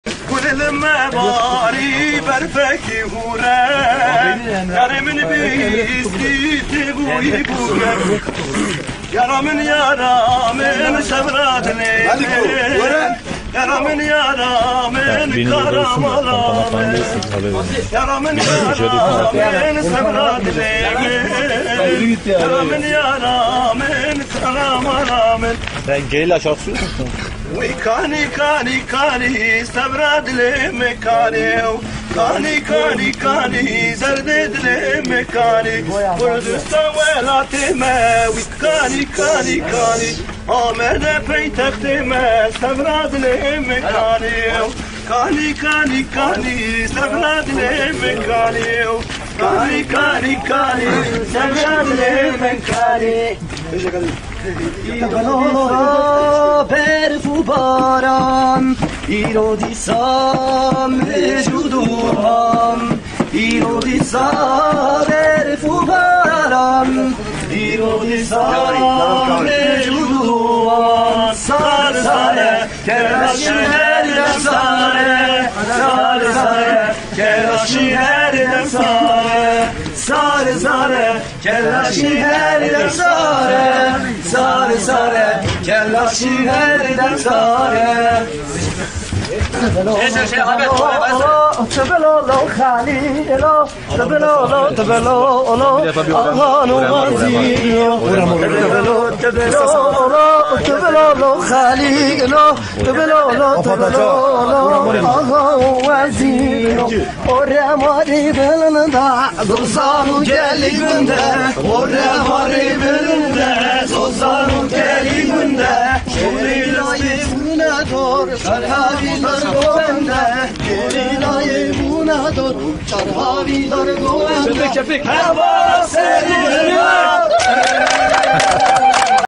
Kürtçe halay çeken askerlere terör soruşturması
Kışlada Kürtçe şarkıyla halay çeken askerler hakkında 'terör örgütü propagandası' yapmak suçlamasıyla soruşturma başlatıldı.